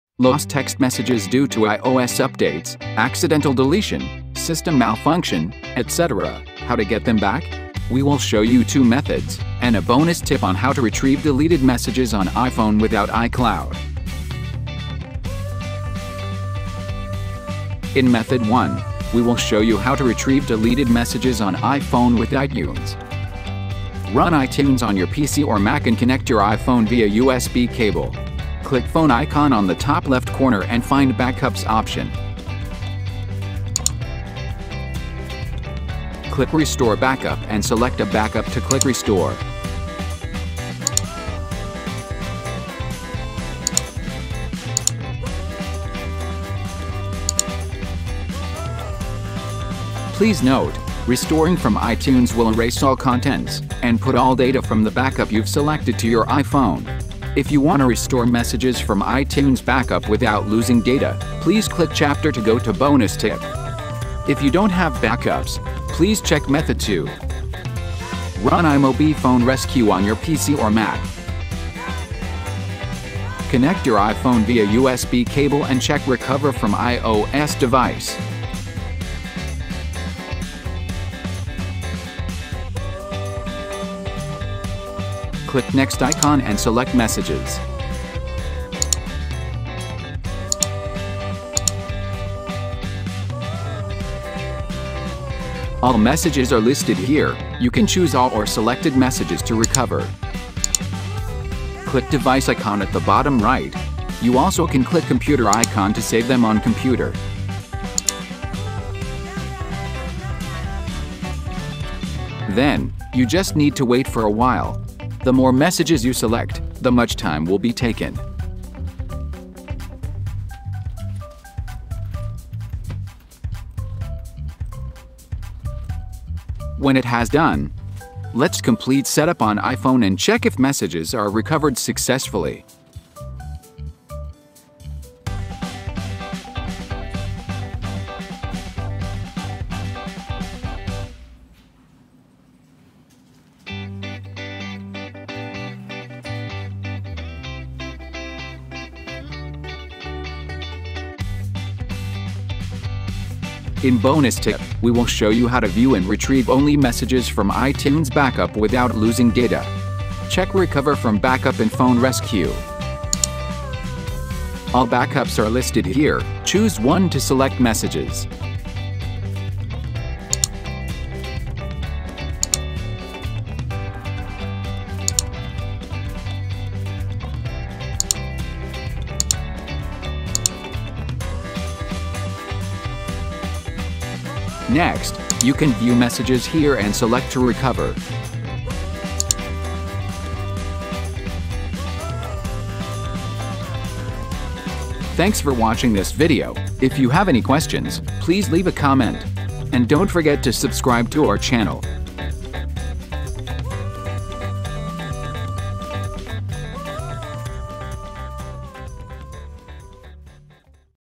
[3 Ways] How To Retrieve Deleted Messages on iPhone without iCloud 2022 | 100% WORKED
Lost text messages due to ios updates, accidental deletion, etc., how to get them back? Check the video on how to retrieve deleted messages on iPhone without iCloud.

Considering some users do not have enough iCloud storage to back up messages regularly, or don't have a habit of backing up iPhone, we will show you 2 methods and a bonus tip on how to retrieve deleted messages on iPhone without iCloud or backup.
00:00 Intro
00:19 Method 1. Retrieve deleted messages on iPhone with iTunes for free
01:04 Method 2. Retrieve deleted messages on iPhone without backup
02:35 Bonus Tip. View and restore only messages from iTunes backup without losing data